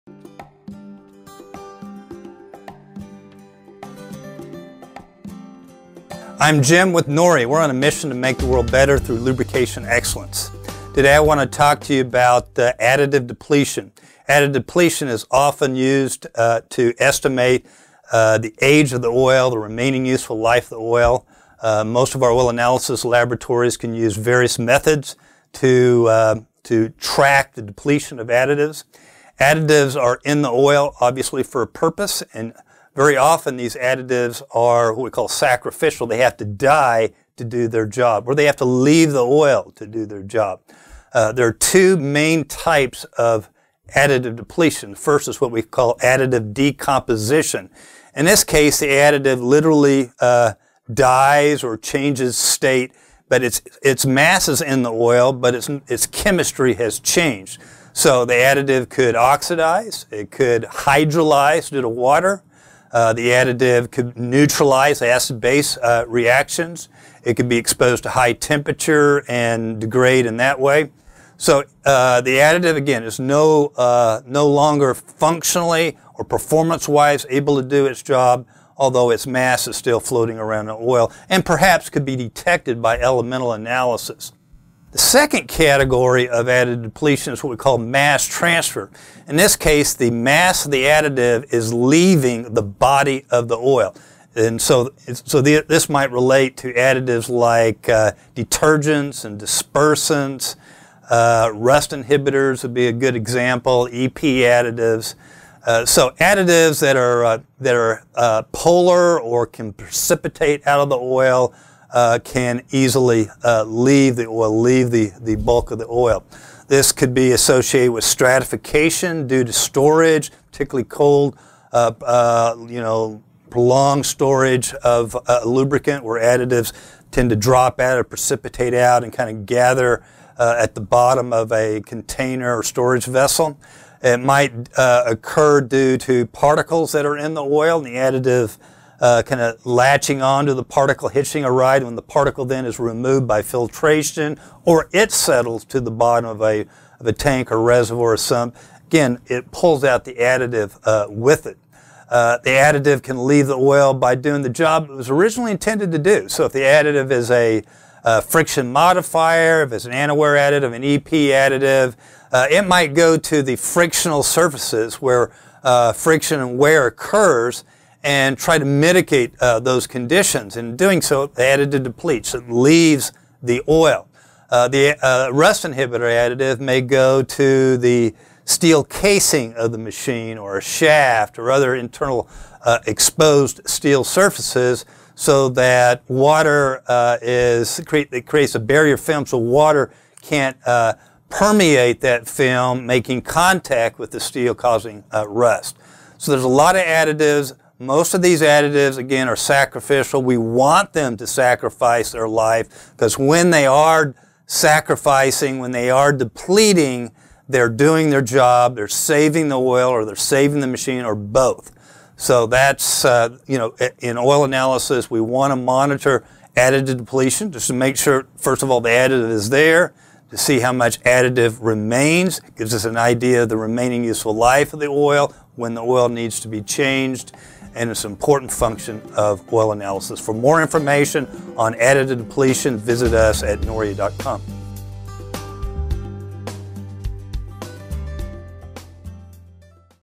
The Truth About Additive Depletion
In oil analysis, additive depletion is often used to estimate the remaining useful life of the oil. Noria expert Jim Fitch explains the two main types of additive depletion and why it is important for additives to deplete in order to save the oil, the machine or both.

Take the next step. Learn how to recognize additive depletion and degradation by attending Noria’s Oil Analysis training courses